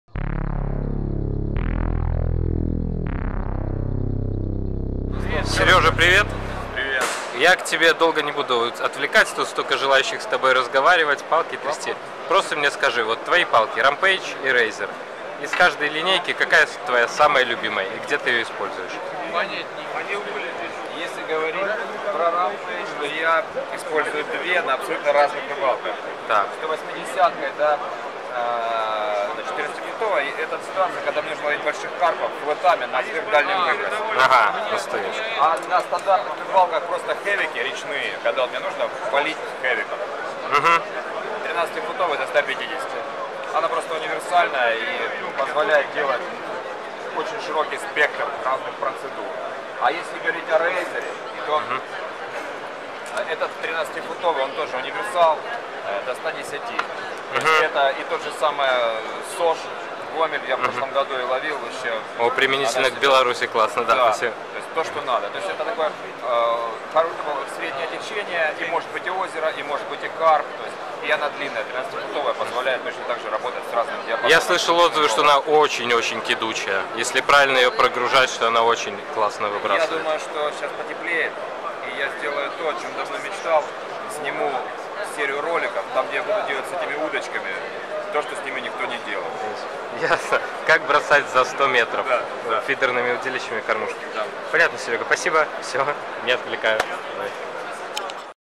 Сергей Попов о любимых фидерах Zemex
Сергей Попов рассказывает о своих любимых фидерах Zemex из линеек Razer и Rampage, разработчиком которых он сам и является.